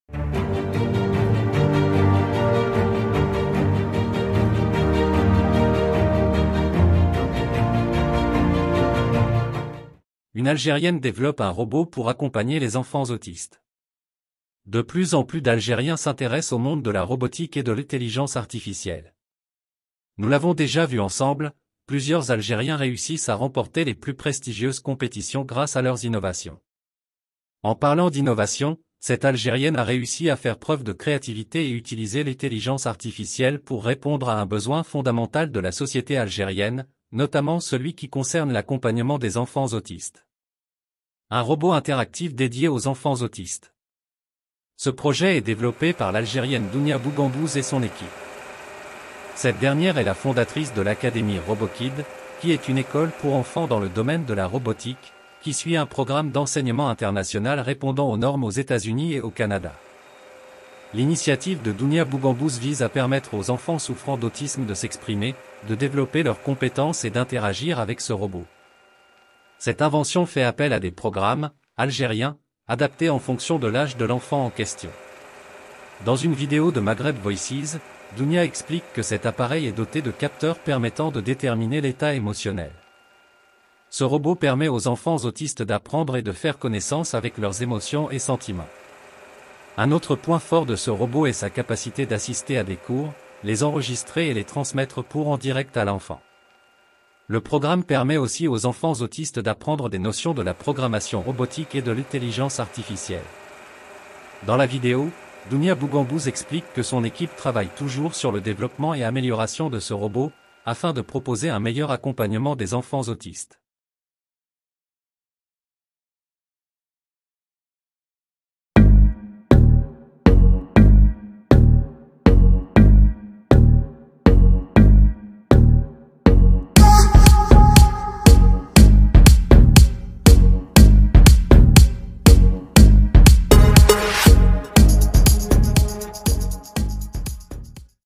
une algérienne developpe un robot 👍👍
De plus en plus d’Algériens s’intéressent au monde de la robotique et de l’intelligence artificielle. Nous l’avons déjà vu ensemble, dans les colonnes d’Algérie360, plusieurs Algériens réussissent à remporter les plus prestigieuses compétitions grâce à leurs innovations.